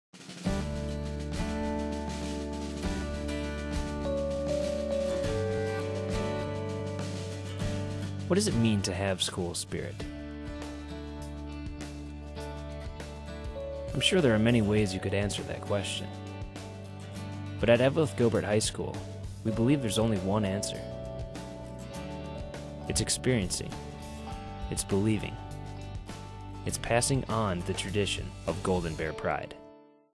Eveleth Gilbert - Tradition
This is a commercial I created for the Eveleth Gilbert Public School District.
(Inspired by my own High School experiences.)

*Special thanks to the people of Eveleth for being so wonderful in front of the camera.